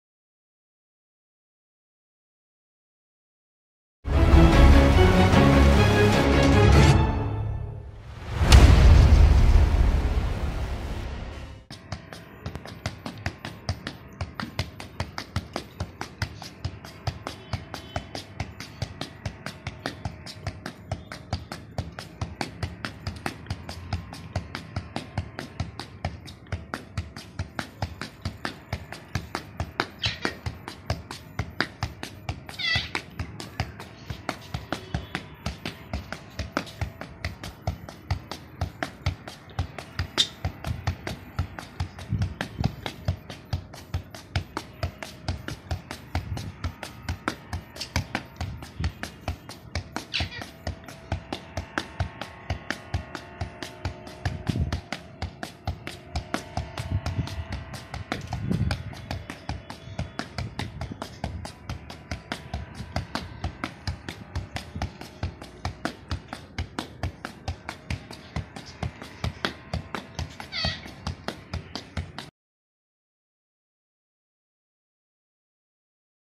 MAXIMUM FOOTBALL JUGGLES WITH BOTH FEET IN ONE MINUTE
The record for doing the maximum number of football juggles with both feet was set by Karunavataram Dhyani (born on February 17, 2014) of Ghaziabad, Uttar Pradesh. He did 137 football juggles (without dropping the ball) in 1 minute at the young age of 6 years, 4 months, and 5 days, as confirmed on June 22, 2020.